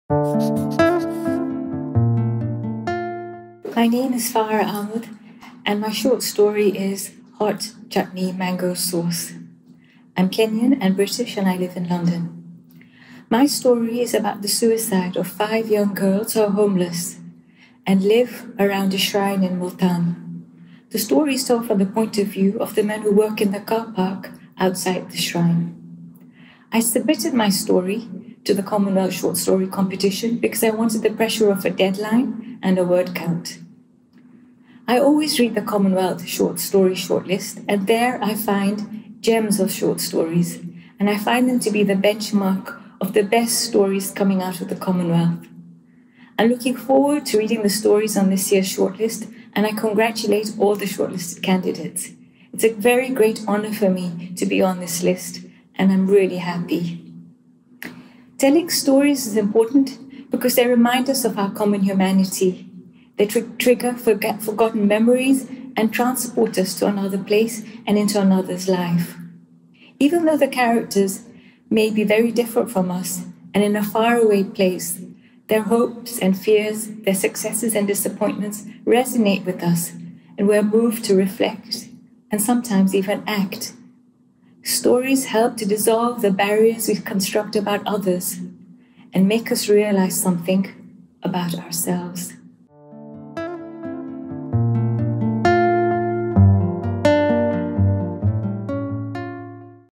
Meet the 2022 Shortlist: Farah Ahamed
Farah Ahamed (United Kingdom/Kenya), shortlisted story: ‘Hot Chutney Mango Sauce’

Find out more about Farah and all of 2022’s shortlisted writers and their stories